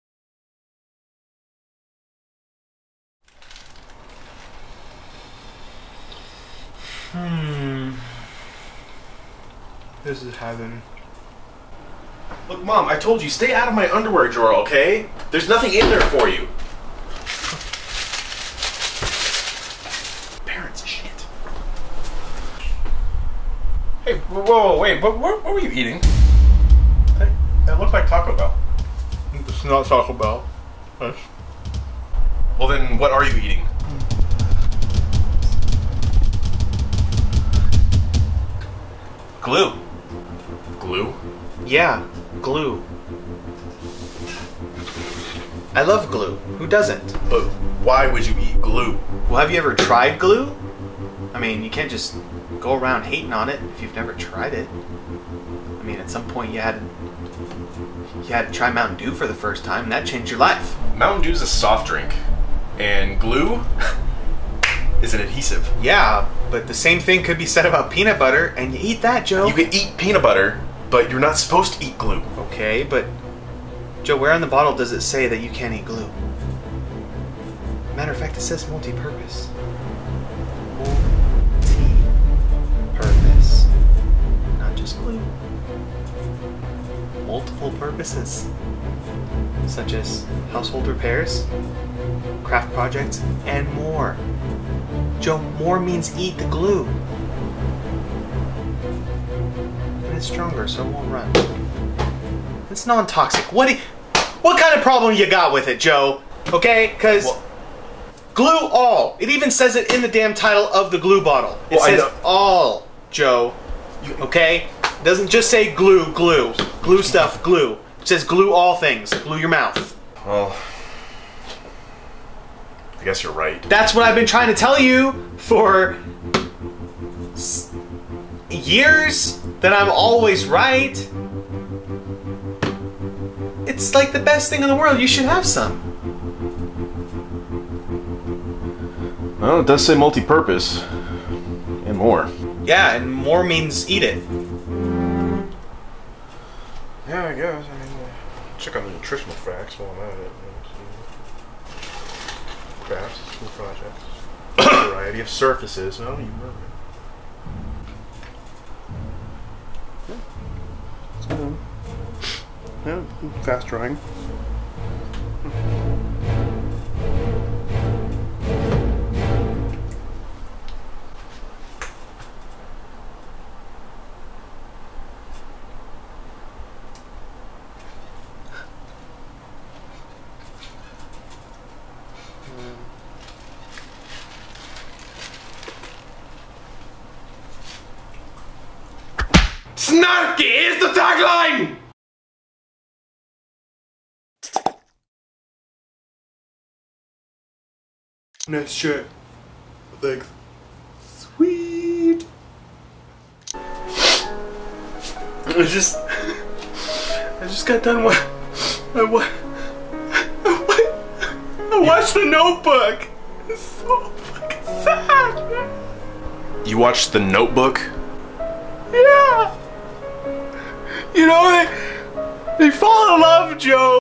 Taco Bell Burrito Defense #1
No one likes to give away free Taco Bell, it is too delicious and cheesy. In hopes to foil Joe's impending plan to have some of Martin's Taco Bell, Martin uses his jedi mind tricks to make Joe eat glue

Cre